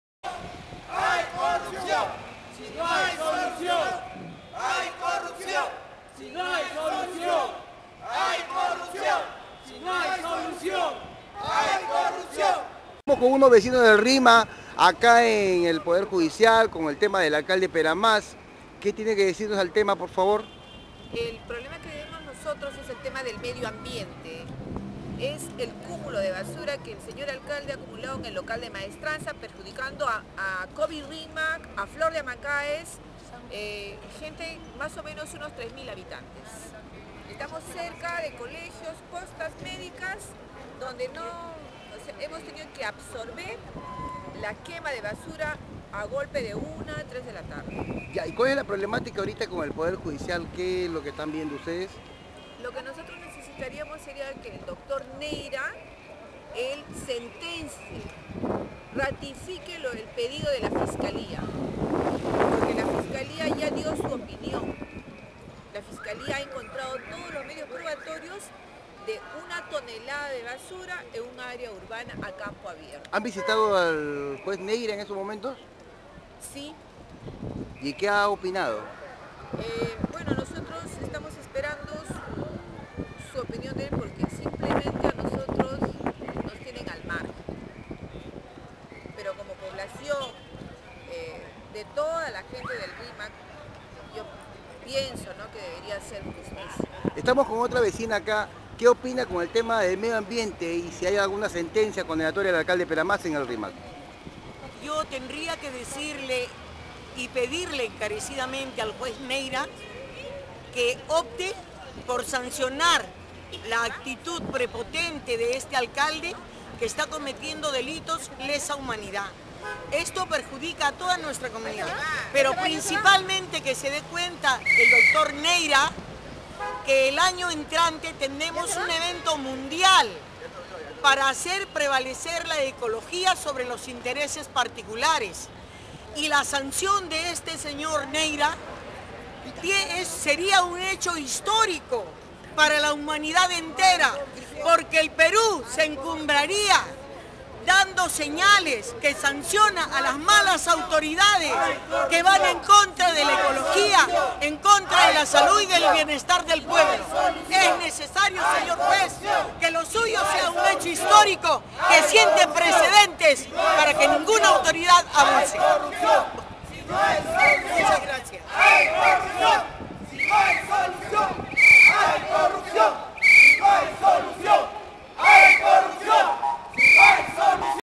Vecinos Rimenses, Protestan En Juzgado Penal De Lima.
15 de  octubre  del  2013
Juez Rodolfo Neyra, Del 35 Juzgado  Penal  Del Lima, Dilata  Sentencia, Contra  Alcalde  Enrique  Peramas
Los  Vecinos  Del  Distrito  De  Flor  D E   Amancaes   Del  Rímac, Protestaron En  El  35  Juzgado  Penal  De  Lima  , Denunciando  Que  El Juez  Rodolfo  Neyra Rojas ,  Viene   Dilatando   La  Sentencia     Contra  El  Alcalde   Enrique  Peramas   , Al  Cual  La  Fiscalía   Ya   Entrego  Las  Pruebas Condenatorias    Del  Delito  Contra   El  Medio  Ambiente.
Vecinos   Pidieron  ,  Al  Juez Que  De  Solución A  La  Sentencia ,Si  No Se  Estaría  Prestando Para   La  Corrupción  Y  Seria  Denunciado También.

Jefe  de  prensa
Carlos  cavassa